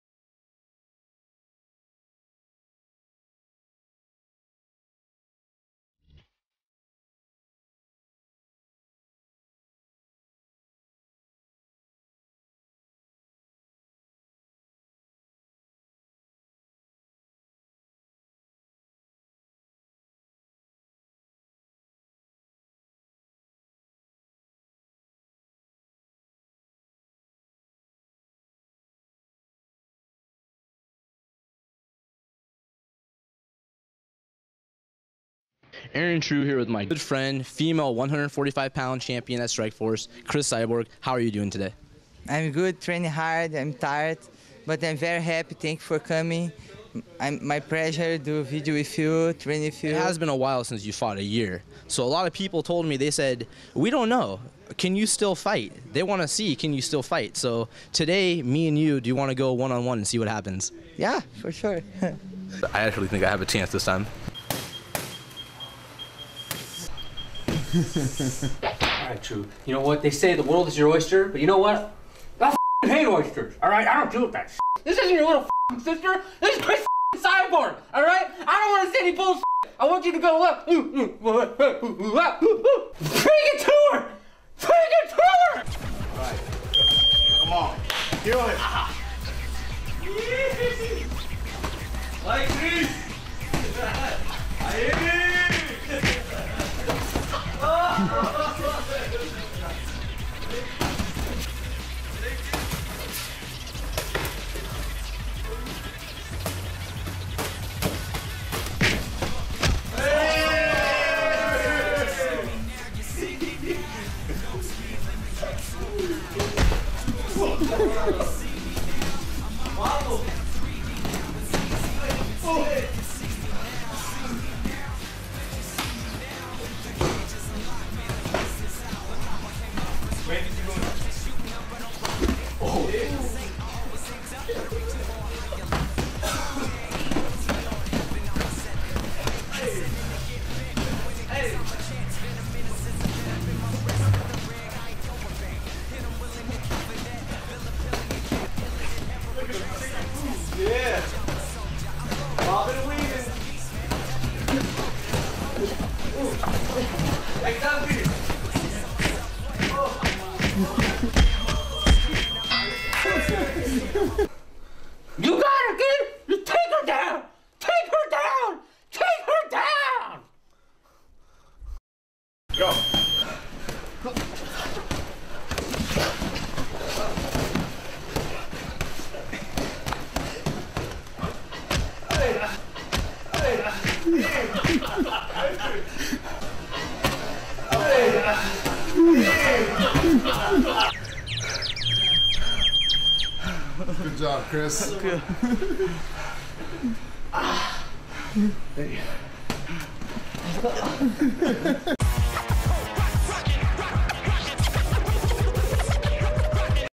Cris Cyborg KO Reporter
StrikeForce Female Champion Cris Cyborg is once again challenged by her friend and reporter Aaron Tru, to a fight.  This time, she wears toy gloves and head stomps are legal.
Video features @AaronTru and @CrisCyborg music by MC IMPRINT